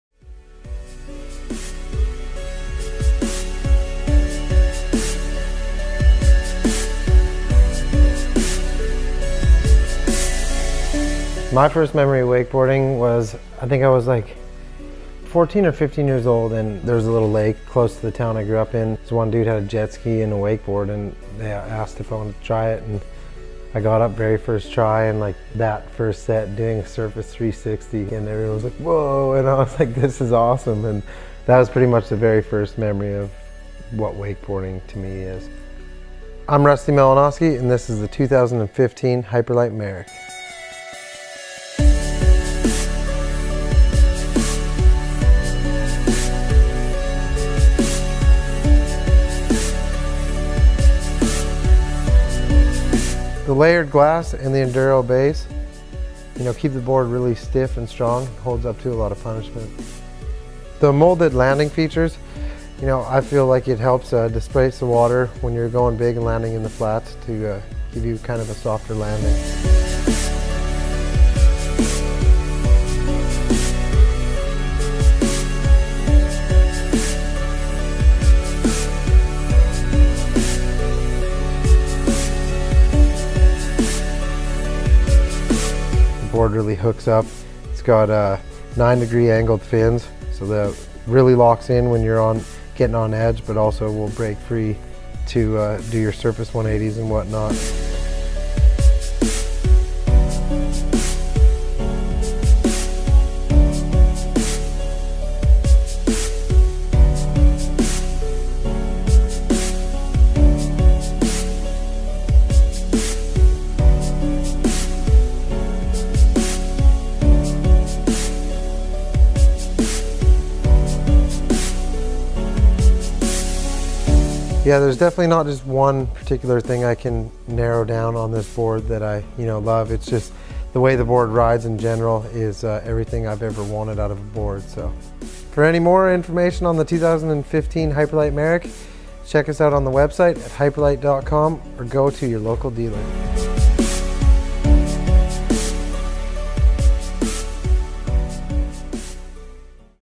Hyperlite Marek Rusty Malinoski Pro Model Wakeboard HD
Speed, Response and Pop, three of Rusty’s favorite things. Bright and bolder than ever.
Designing a shape to carry the Marek name was a process that took months of development. Beginning with the overall volume of the board, now reduced by 15%, the Marek is lighter than the original generation. Maintaining a responsive edge and speed into the wake was key in the new design. The Abrupt Continuous rocker line produces great pop off the wake while allowing the board to maintain the quick feel it has been famous for. The next generation Marek features slightly mellower base features resulting in a quicker release off the wake, allowing a rider to initiate a trick sooner. The Marek is everything you have come to love about Rusty’s shapes in a leaner more responsive package.
CONSTRUCTION FEATURES
• Biolite 3 Core
• Enduro Base
• Abrupt Continuous Rocker
• Two Stage Channel
• 9 Degree Angled Fins
• Molded Landing Feature
• Monocoque Construction
• Layered Glass
• M6 Inserts